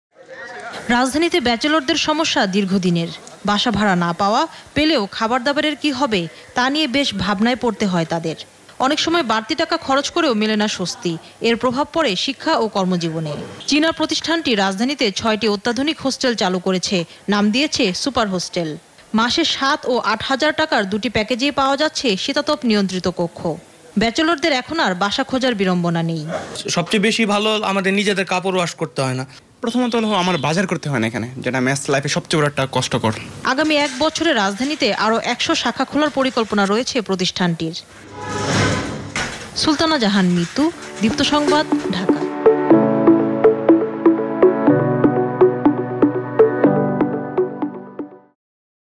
মাসে মাত্র ৬,৯৯৯ টাকায় ৩ বেলা খাওয়া, এসি রুমসহ ৩০টিরও বেশি সুবিধা | 1/3/7/15 days & monthly
আপনি কি চাকুরীজীবী/ স্টুডেন্টস/ ডাক্তার/ ব্যাংকার, বাসা খুঁজতে খুঁজতে ক্লান্ত ? আপনার জন্য রয়েছে "সুপার হোম"। ৩ বেলা খাওয়া, এসি রুম, লন্ড্রি সার্ভিস, রিডিং রুম সহ ৩০টিরও বেশি সুবিধা।

মান্থলি প্যাকেজের পাশাপাশি রয়েছে ১/৩/৭/১৫ দিনের প্যাকেজ।

উত্তরা, মিরপুর, বাড্ডা, বারিধারা ও শাহবাগে ছেলেদের এবং মিরপুরে একটি মেয়েদের জন্য রয়েছে সুপার হোমের" শাখা। ঝামেলাহীন জীবন যাপনে আপনার অপেক্ষায়। এখানেও রয়েছে ২/৩/৪ জনের রেগুলার রুম, এবং মাল্টিফাংশনাল বেডিং।
1 Day Package- 699 Tk. ( No extra fee needed)
3 Days Package-1,799 Tk. ( No extra fee needed)
7 Days Package- 2,999 Tk. ( No extra fee needed)
15 Days Package- 4,999 Tk. ( No extra fee needed)
1 Day Package- 799 Tk. ( No extra fee needed)
3 Days Package-2,099 Tk. ( No extra fee needed)
7 Days Package- 3,999 Tk. ( No extra fee needed)
15 Days Package- 5,999 Tk. ( No extra fee needed)
1 Day Package- 999 Tk. ( No extra fee needed)
3 Days Package- 2,699 Tk. ( No extra fee needed)
7 Days Package- 4,999 Tk. ( No extra fee needed)
15 Days Package- 6,999 Tk. ( No extra fee needed)